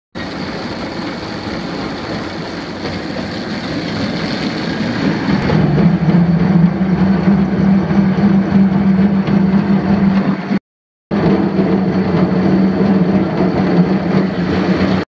Automatic ATGA900 roller mill for sale at auction | bidding closes January 19, 2022
Automatic ATGA900  roller mill
 540 PTO
 Auger Diameter: 10"
 Tire: 205/75R15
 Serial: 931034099